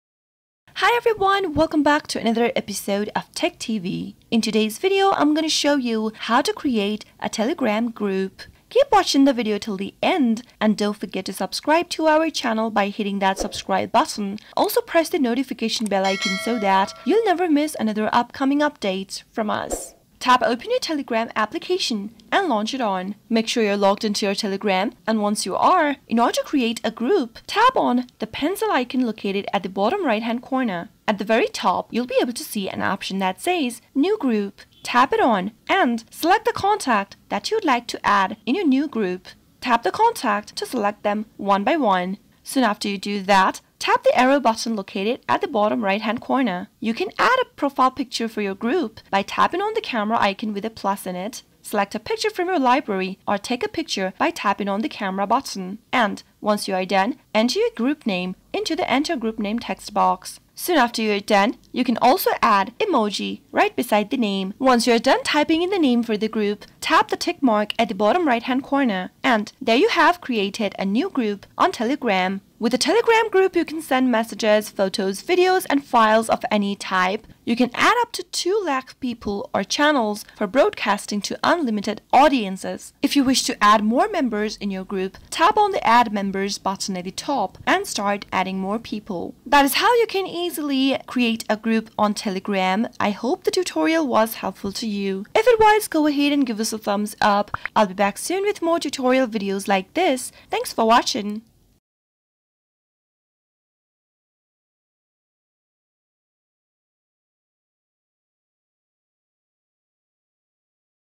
How to Create a Group in Telegram | Create Telegram Group 2022
In this video I am going to guide you How to Create a Group in Telegram.

CHAPTERS
0:00 - Intro
0:19 - How to Create a Group in Telegram
1:32 - Outro

Hey EveryOne!